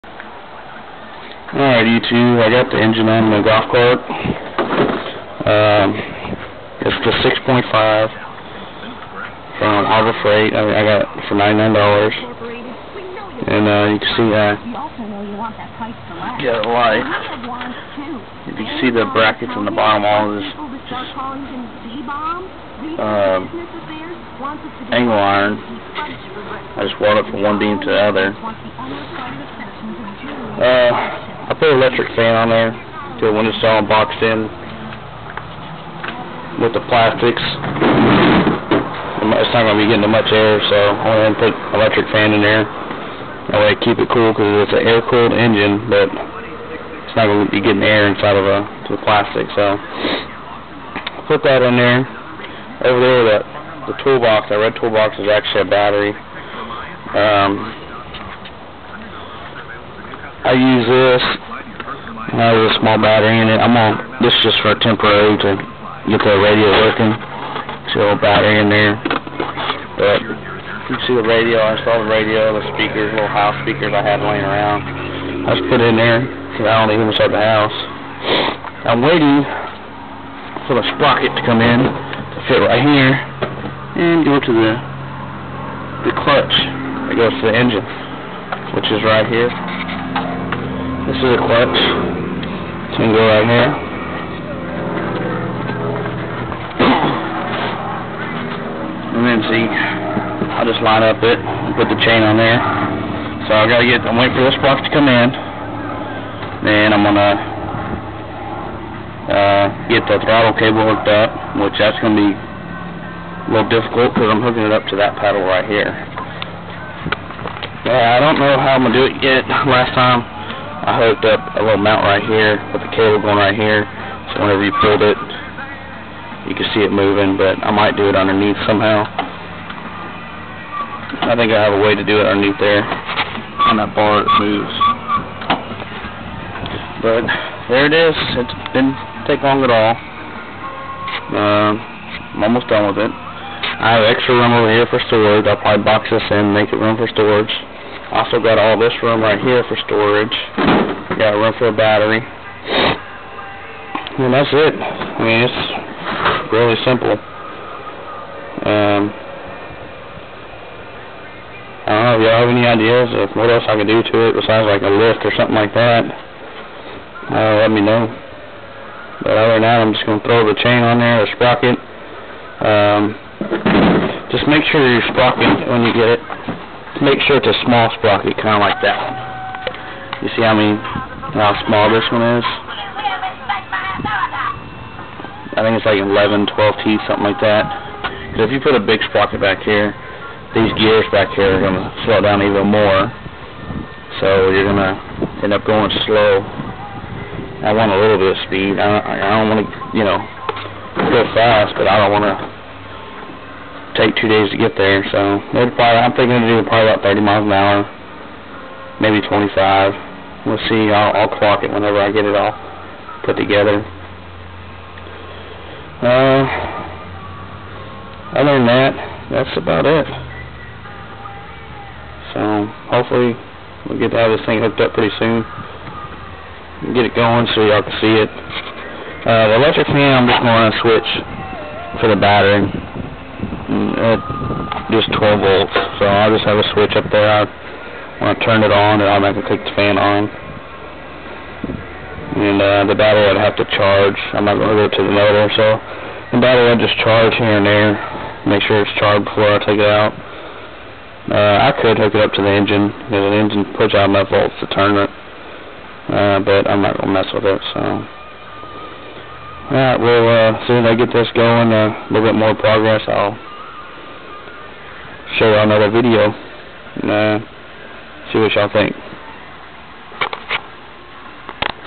Electric gold cart to gas. How to put gas motor on electric cart pt. 2
Here is the motor on the cart. Only one problem i have is the seat is touching the engine. So ill have to raise the seat a little.